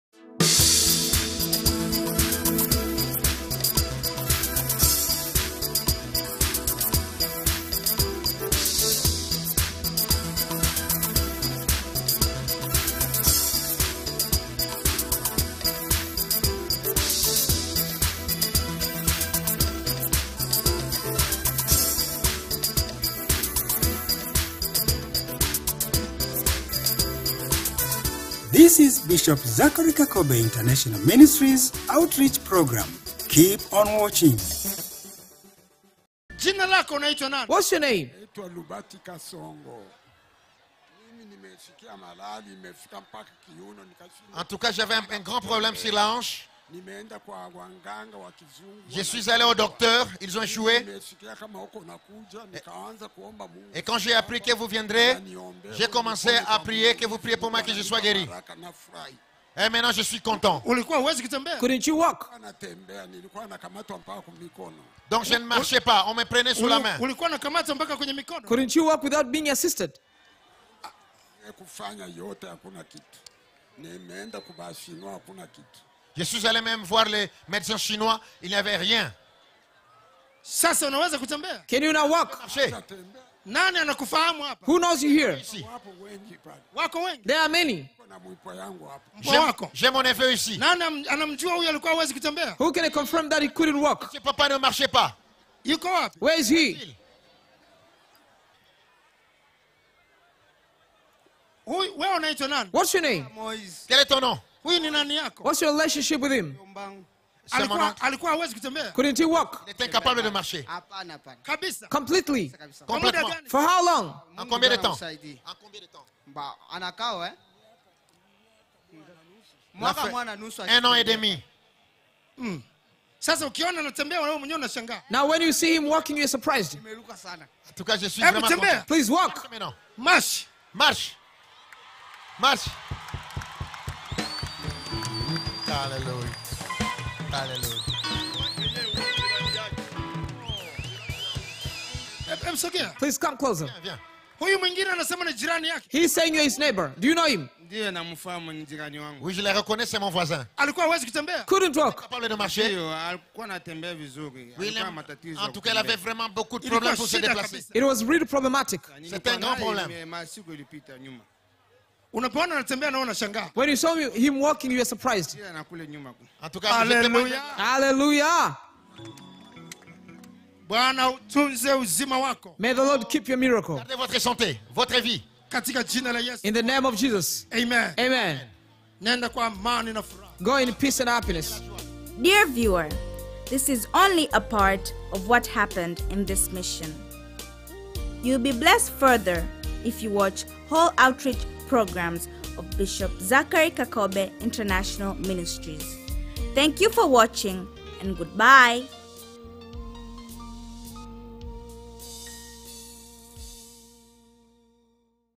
Lame man healed in Bp. Kakobe's Lubumbashi DRC Crusade
The miracles which occured at the Bishop Zachary Kakobe's Lubumbashi-CONGO (D.R.C) Great Miracle Healing Crusade 2010, were countless. This is one of the miracles. Here a man who was lame for about one and a half years, stands up and walks freely, to the amazement of his relatives and neighbours. Watch this clip for more details.